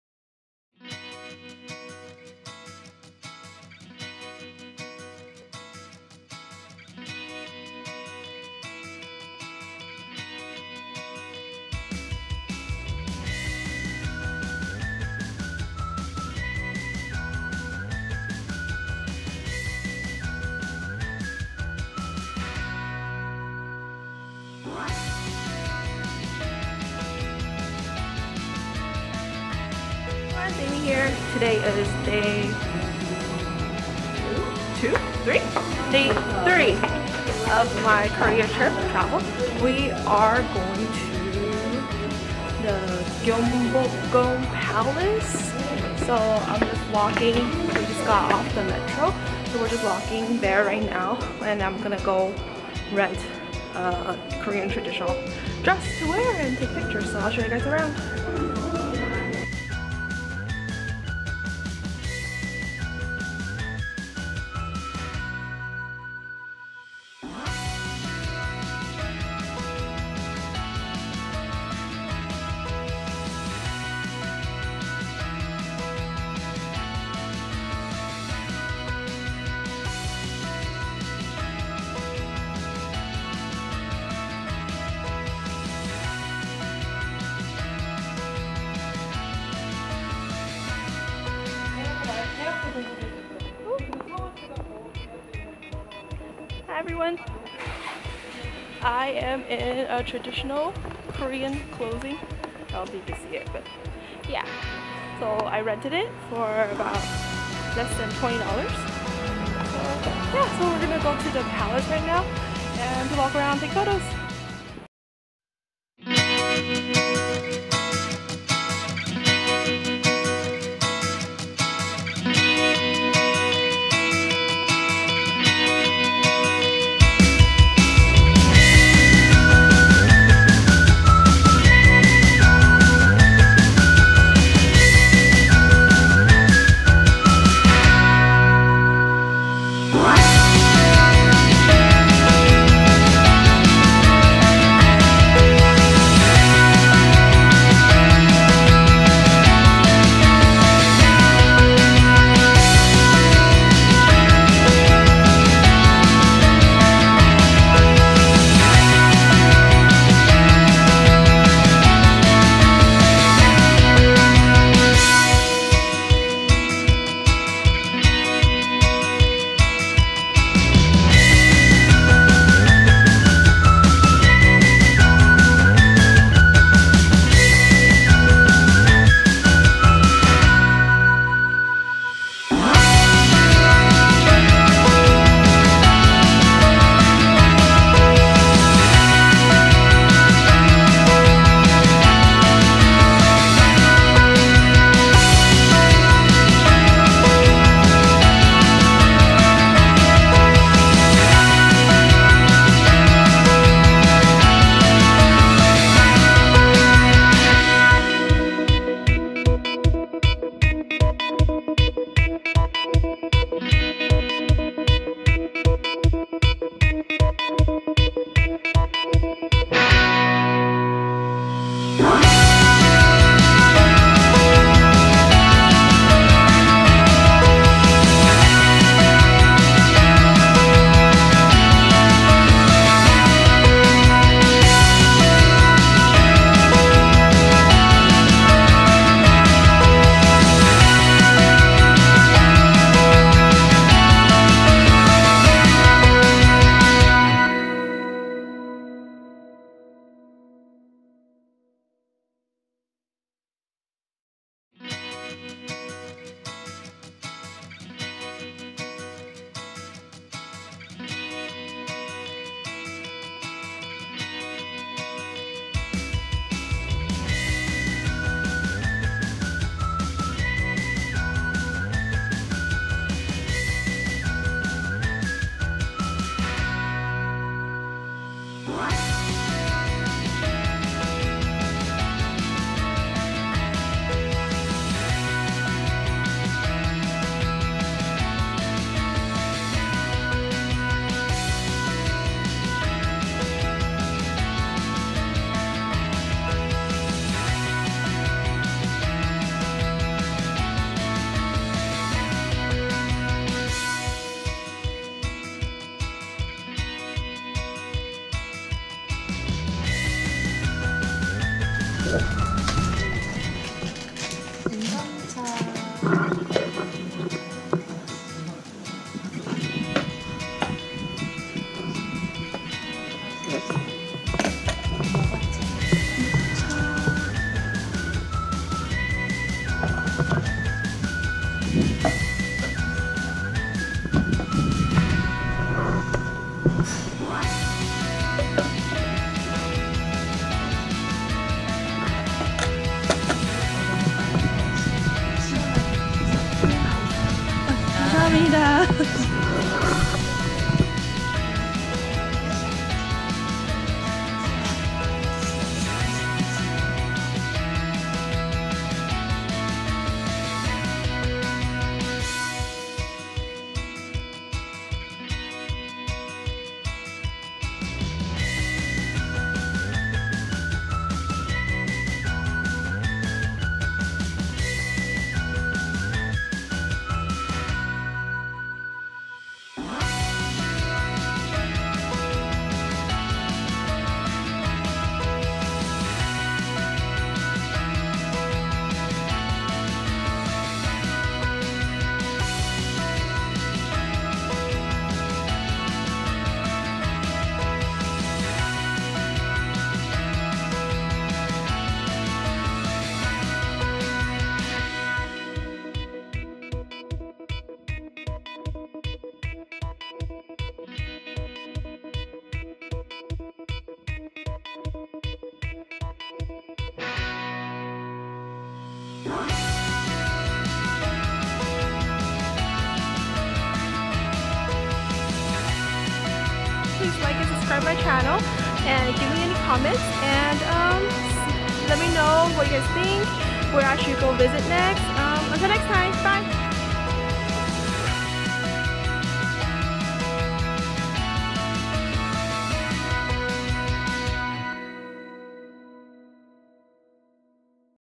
Travel vlog #4 Embracing history at Gyeongbokgung Palace
Spending a day walking through history.

Thanks to the girls at Princess Hanbok for picking this pretty hanbok for me to wear!

Address: 52-6 Anguk-dong, jongno-gu, Seoul